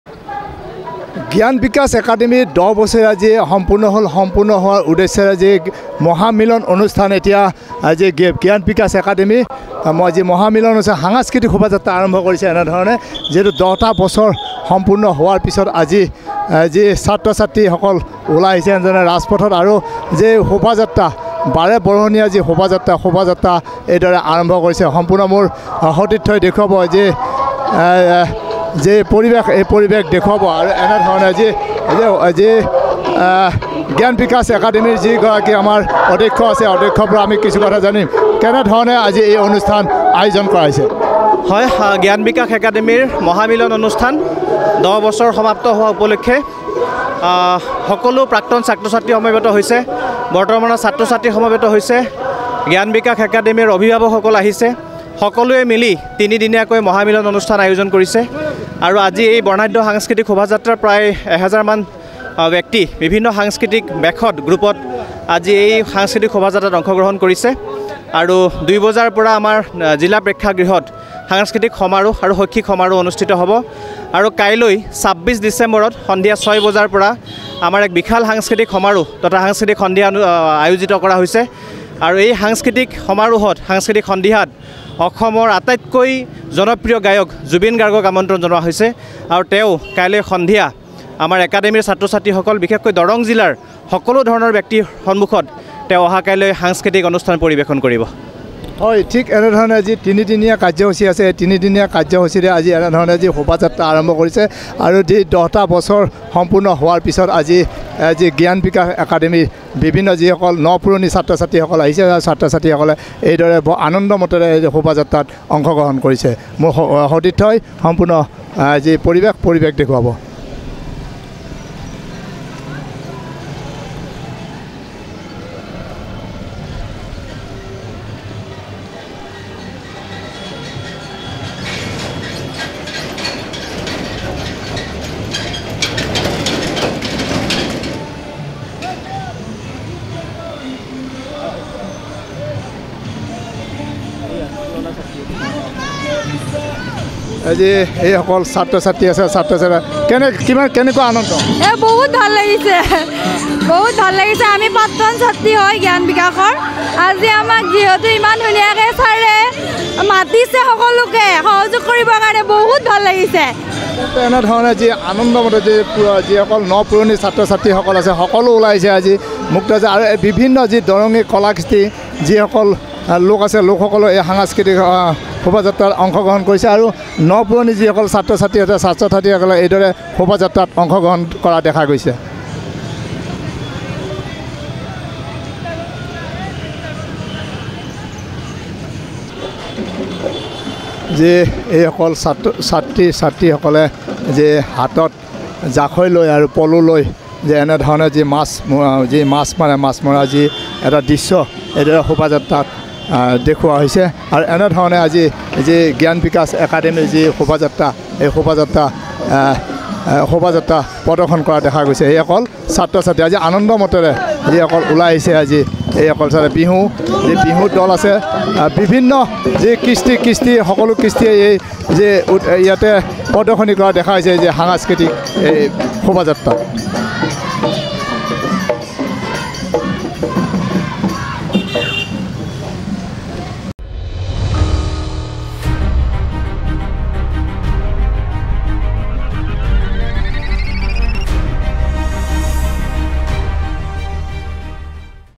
মঙলদৈৰ জ্ঞান-বিকাশ একাডেমিৰ দশম বৰ্ষপূৰ্তি উপলক্ষে মহামিলন অনুষ্ঠান।
মঙলদৈৰ জ্ঞান-বিকাশ একাডেমিৰ দশম বৰ্ষপূৰ্তি উপলক্ষে ন-পুৰণি ছাত্র-ছাত্ৰী, অবিভাৱক, শুভাকাংশীৰ মহামিলন অনুষ্ঠান।